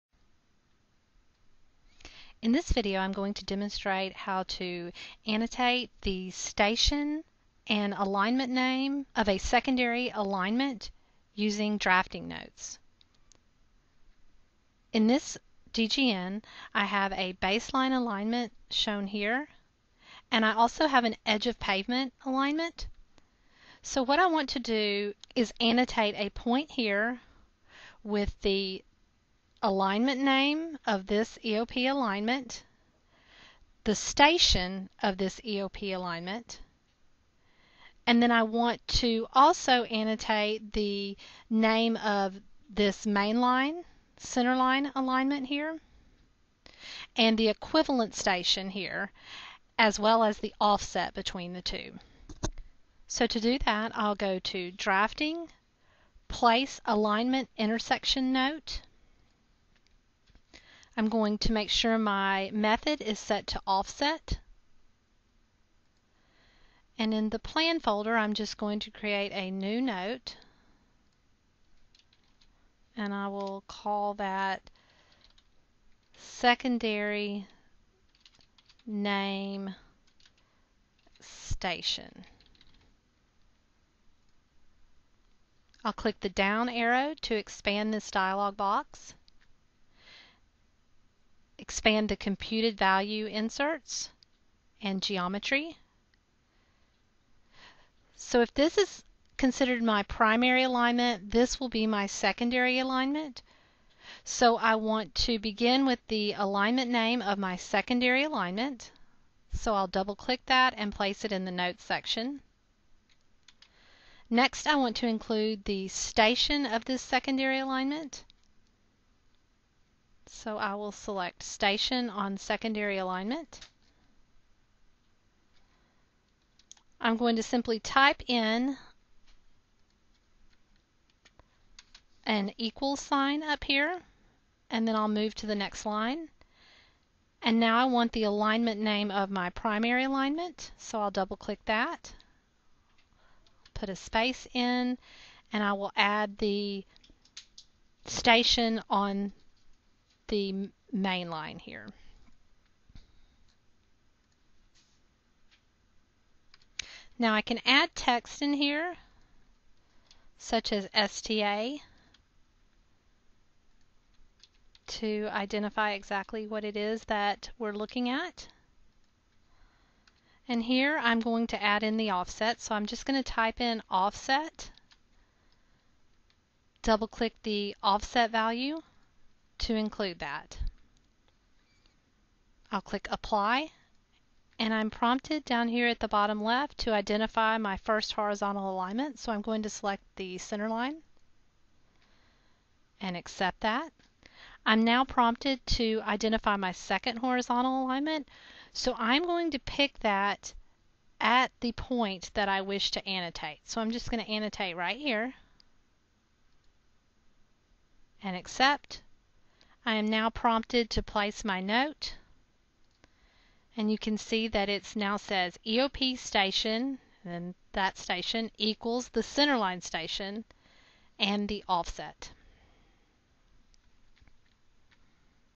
Annotate Name and Station of Secondary Alignment
This video demonstrates how to annotate the name and corresponding station of a secondary alignment using drafting notes.